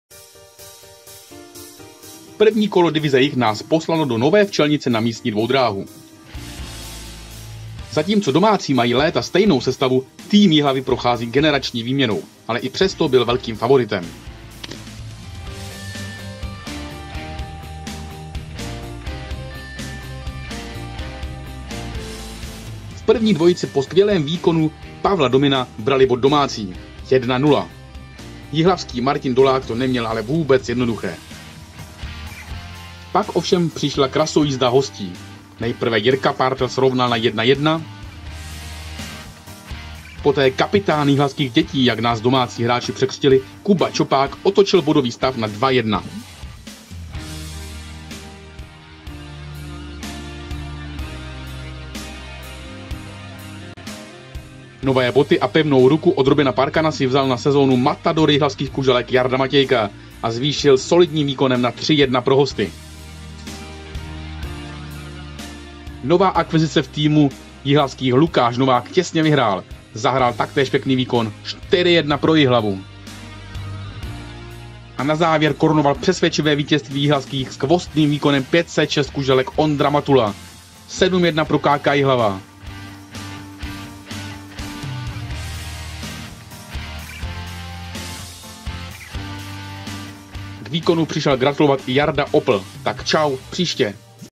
Reportáž z utkání Nová Včelnice - Jihlava 4.9.2020
Reportáž prvního kola kuželkářské Divize Jih.